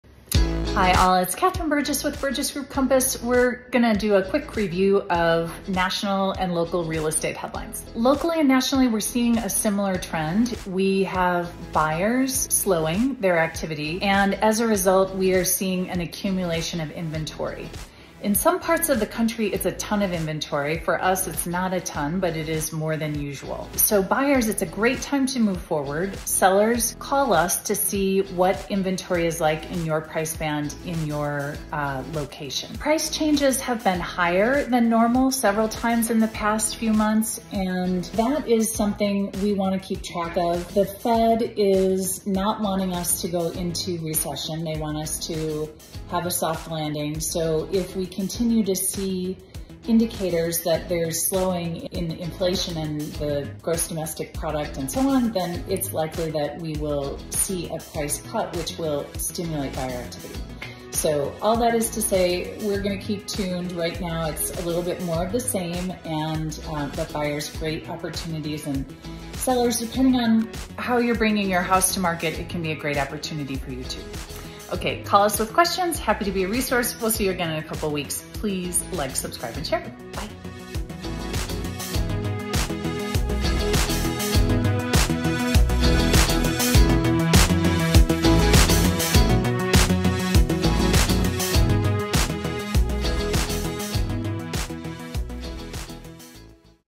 Weekly Market Update, Real Estate Statistics & National News - July 26, 2024
Friends,

Our market has limited inventory and reduced buyer activity. If you find something you like, it's a good time to purchase!

Nationwide, home listings are becoming stagnant as high housing costs suppress demand; simultaneously the supply of homes for sale is increasing.

See your favorite nerdy details below.